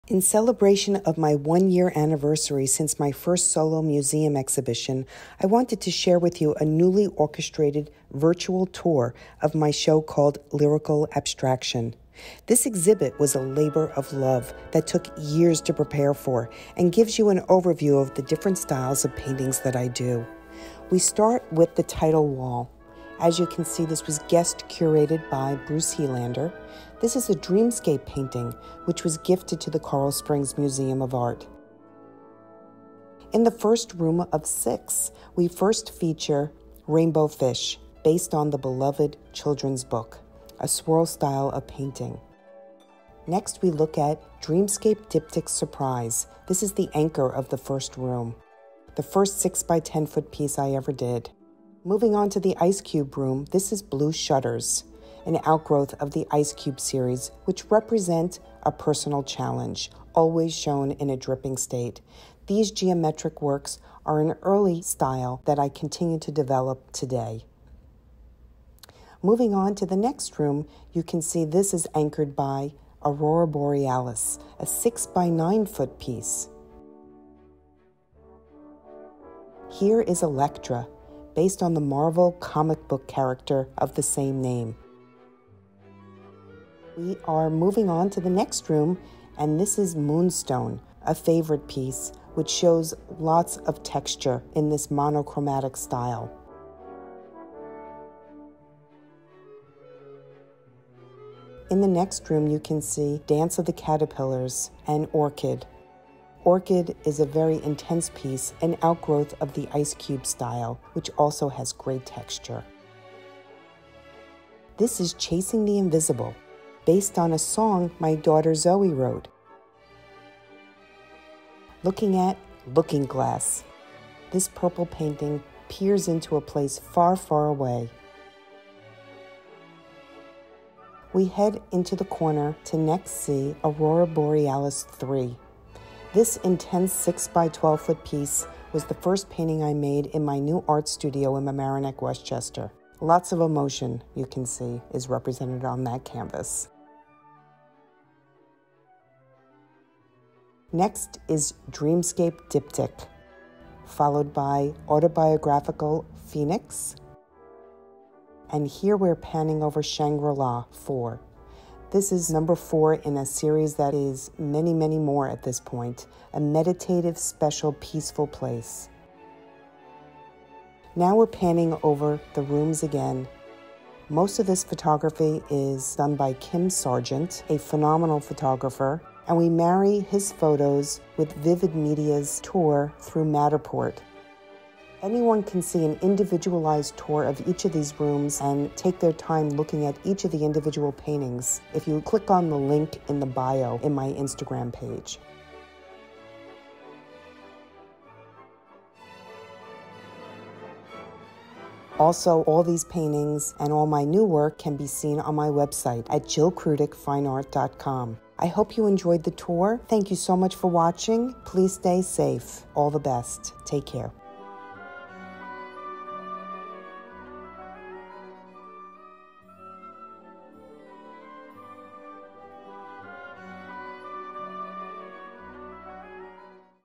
IGTV: Coral Springs Museum of Art: Museum Walkthrough
To celebrate the one year anniversary of Jill Krutick's first solo museum exhibition at the Coral Springs Museum of Art, we have created an enhanced virtual tour video of her Lyrical Abstraction exhibition for your enjoyment.

I want to thank Kim Sargent for his phenomenal photography, VividMediaNY for the Matterport tour, Simone Kurtz for her design and Kathy Mathews for her video editing.